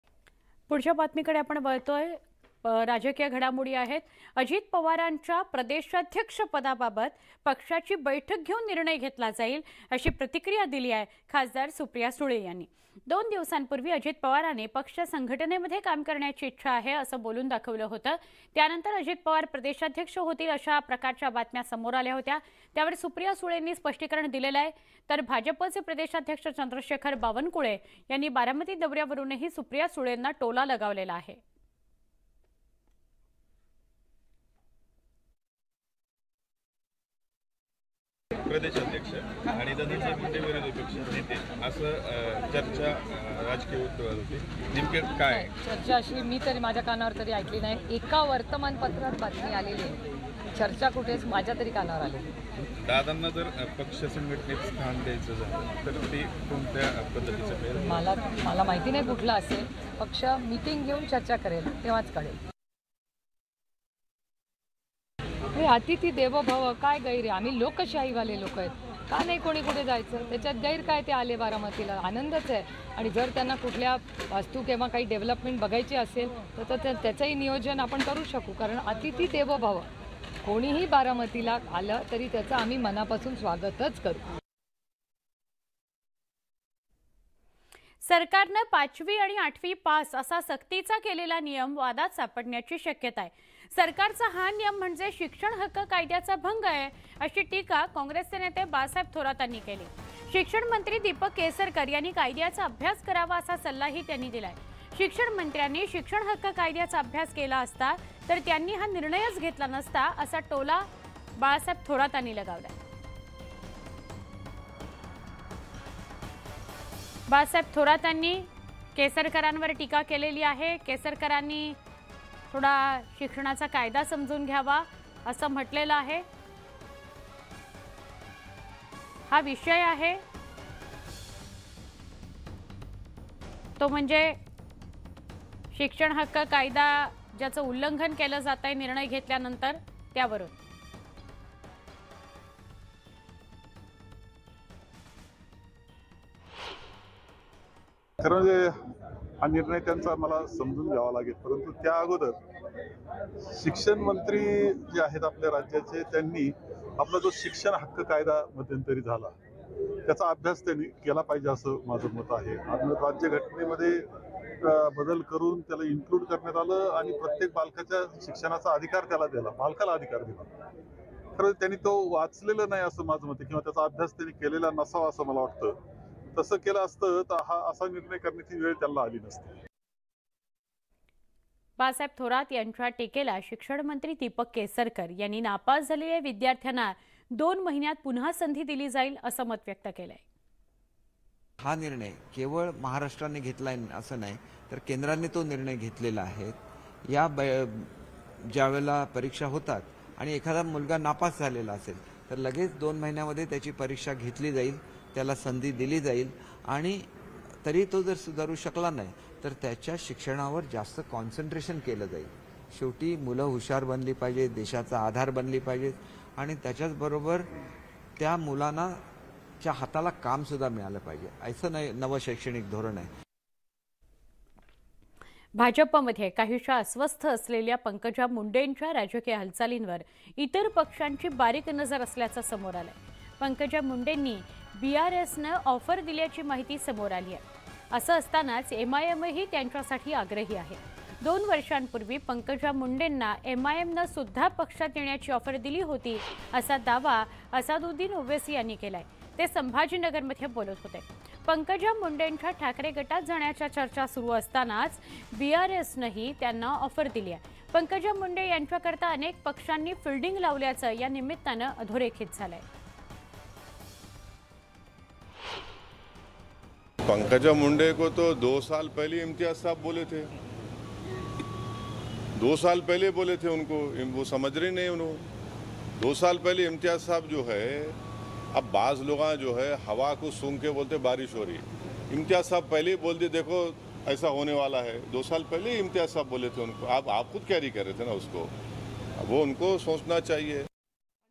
Supriya Sule on Ajit Pawar | अजित पवारांबाबत पक्ष निर्णय घेईल, चर्चांवर सुप्रिया सुळेंचं स्पष्टीकरण
n18oc_news